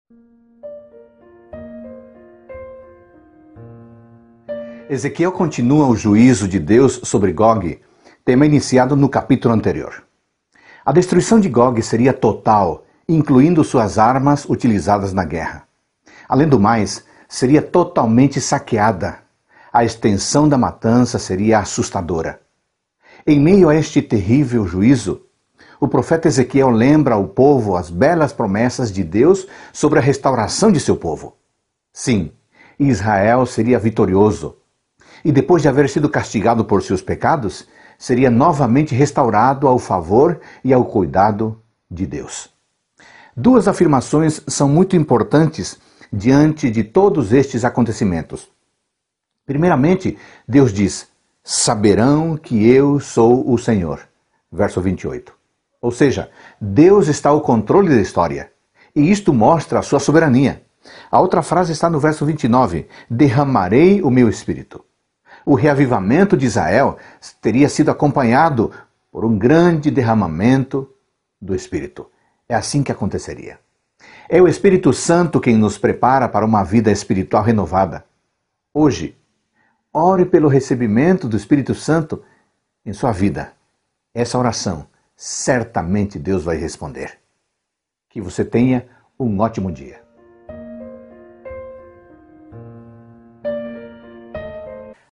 Ezequiel 39 - Reavivados por Sua Palavra
O profeta Ezequiel lembra ao povo as belas promessas de Deus sobre a restauração de Seu povo. Sim, Israel seria vitorioso e novamente restaurado ao favor e cuidado de Deus.